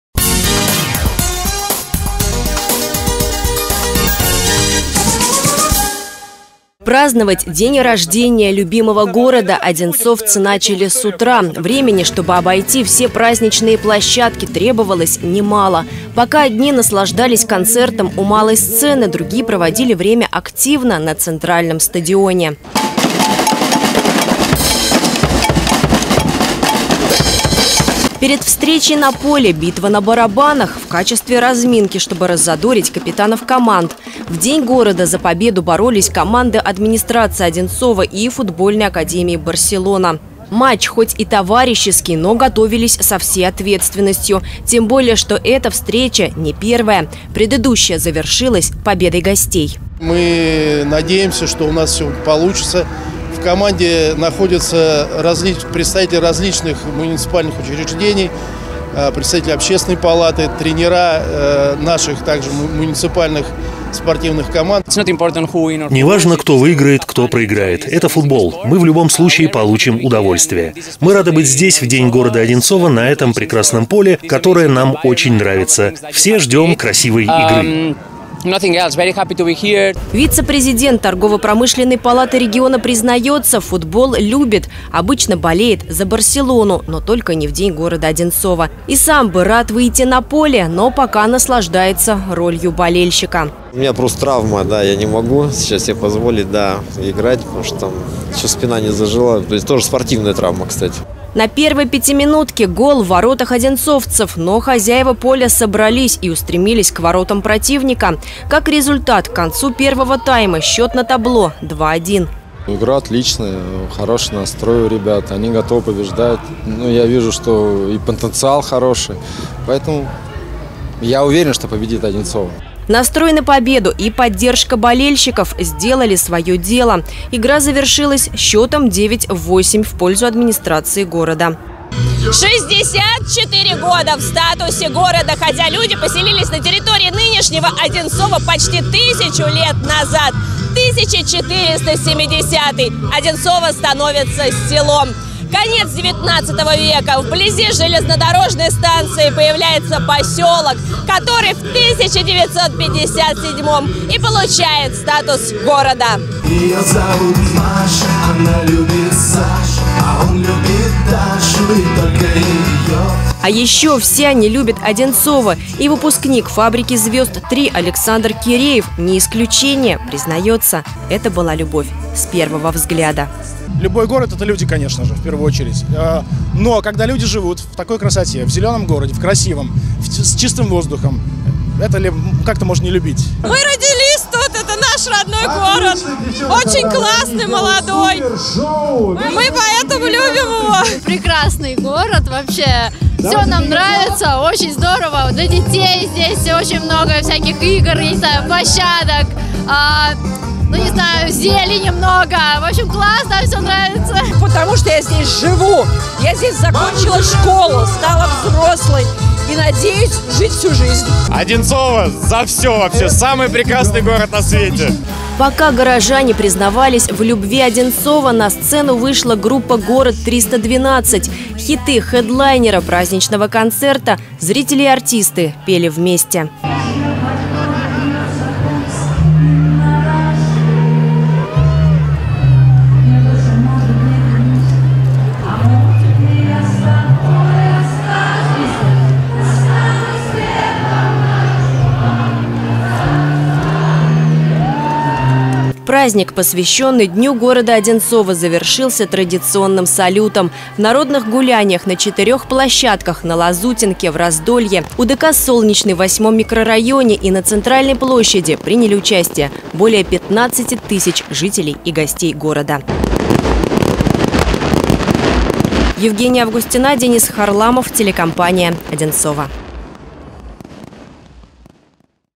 Городу Одинцово – 64!
Город Одинцово отметил свой 64-ой день рождения. Футбольный турнир, праздничный концерт, хэдлайнером которого стала группа «Город-312» и долгожданный салют.